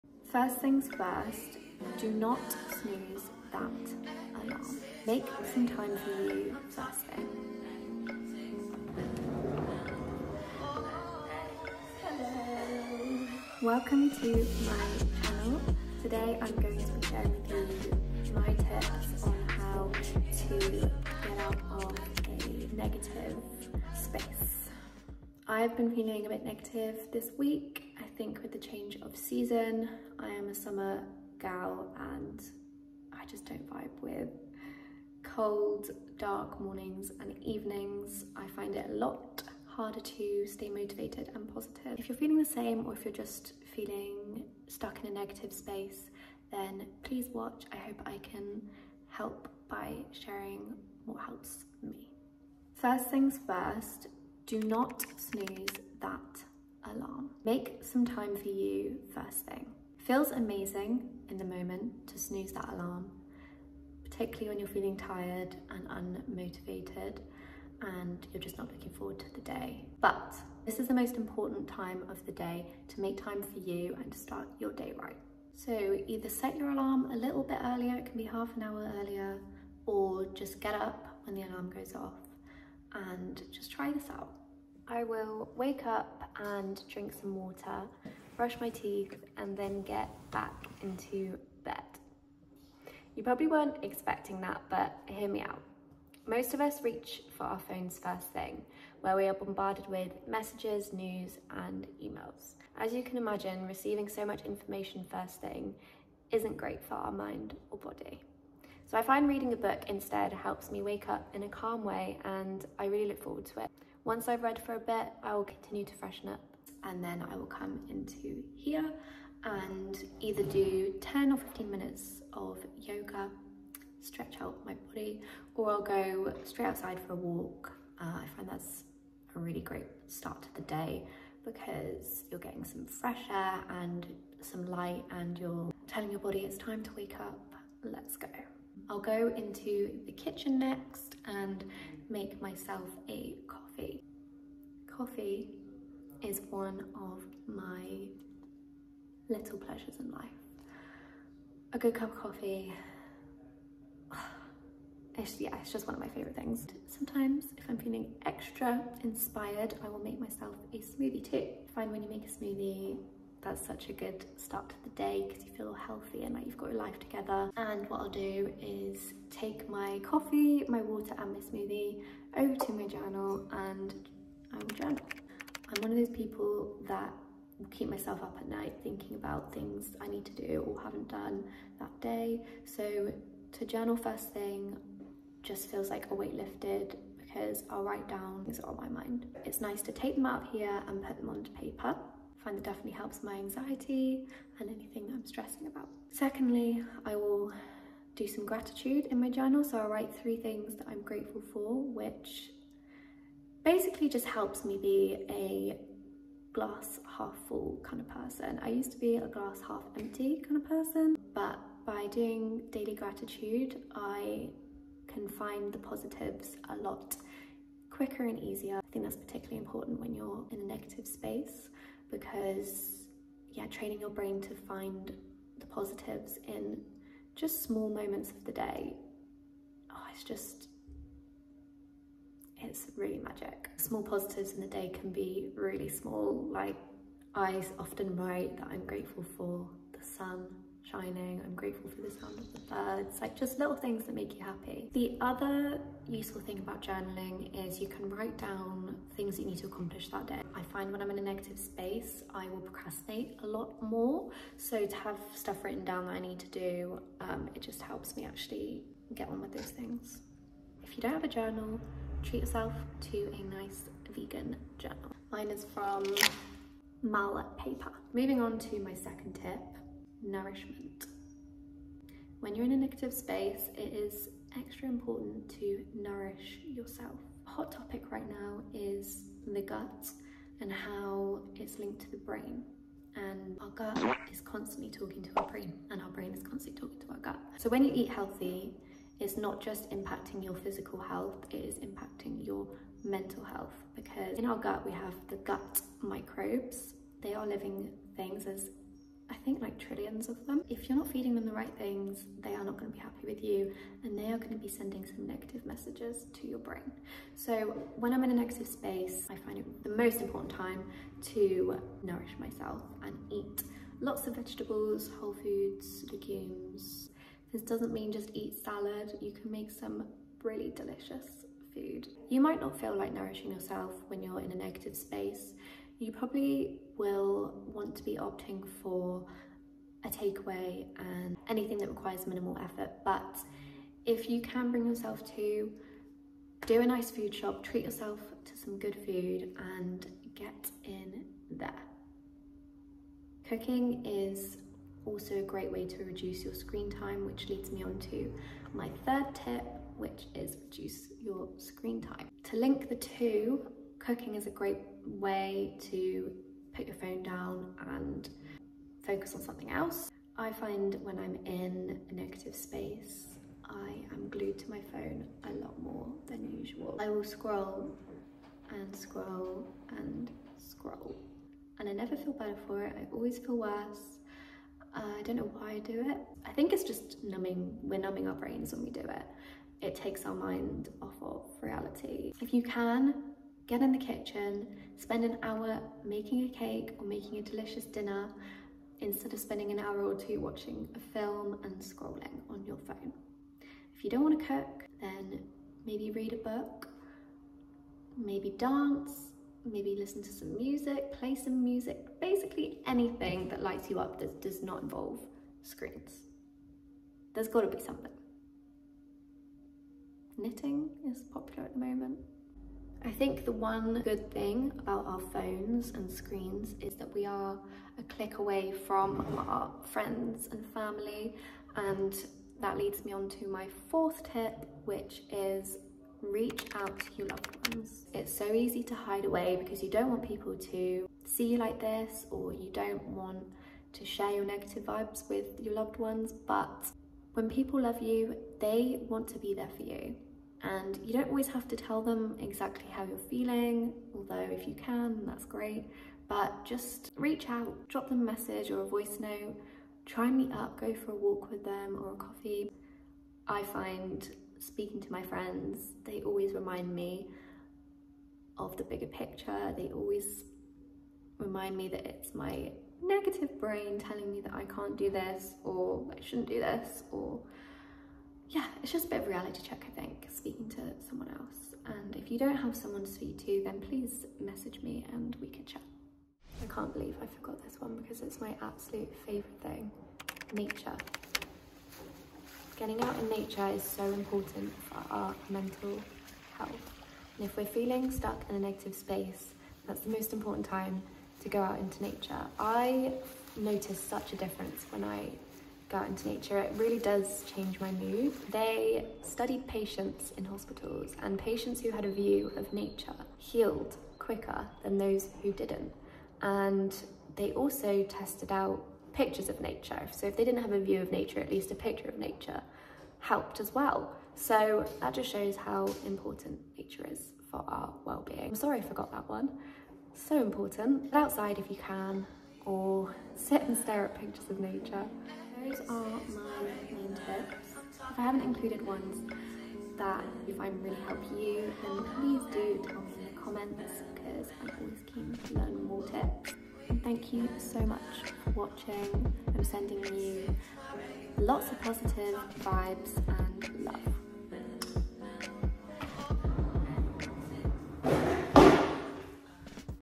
how to get out of a negative space | mental wellness routine
I’ve felt a bit low recently with the change in season (winter h8ter here) ❄️
I know there’s others feeling the same, so thought I’d make a video sharing what helps me to get out of a negative space, in the hopes it can help someone else 🤍